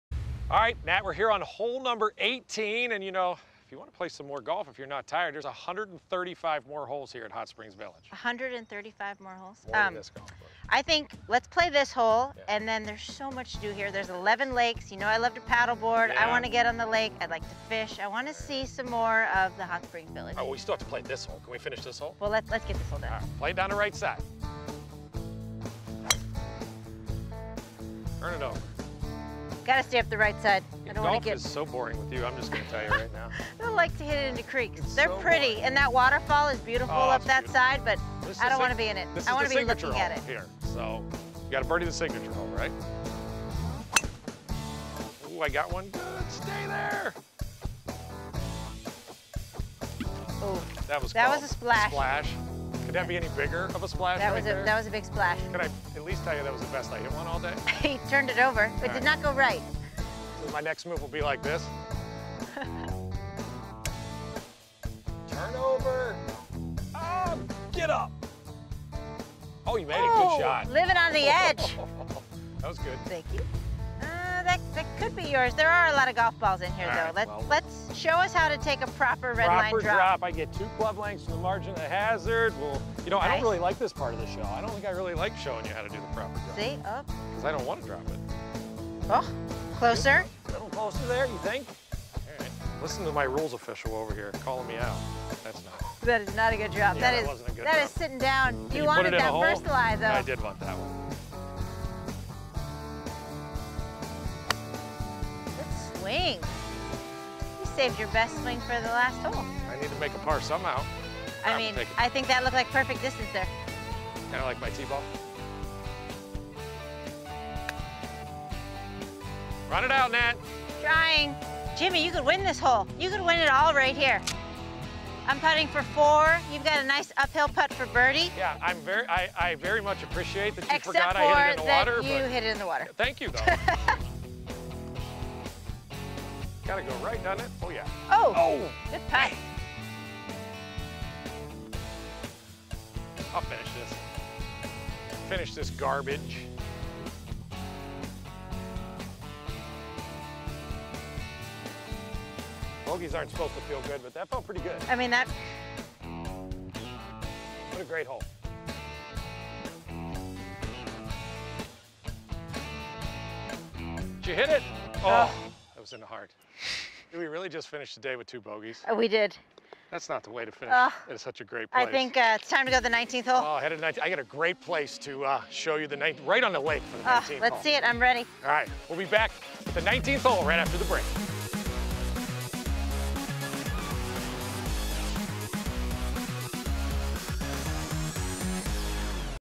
18 Holes - Natalie Gulbis & Jimmy Hanlin take on Aviara
Join LPGA Pro Natalie Gulbis and PGA Pro Jimmy Hanlin as they give you a look at iconic Hole 18 at Aviara Hot Springs Village.